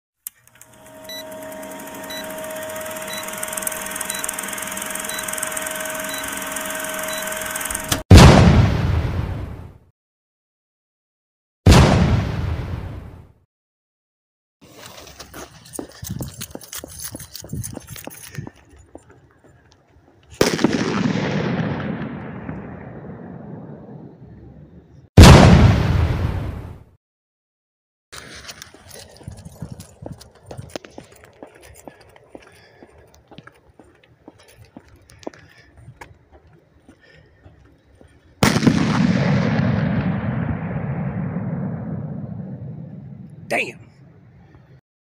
3.5gram Temple of Heaven "Gong Banger" and 10gram "Black Beauty" blockbuster
Some ground salutes for mischief night. 😁